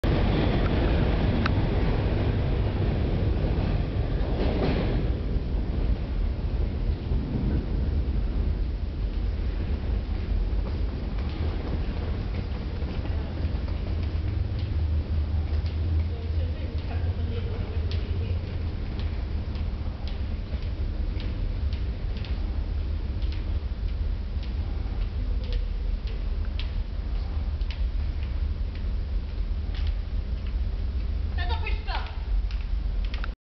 Red Rocket
My photos appeared on transit screens on subway platforms across the city on May 14th, 2007. It was a part of OneStopMedia's CONTACT presentation of Transit Stories.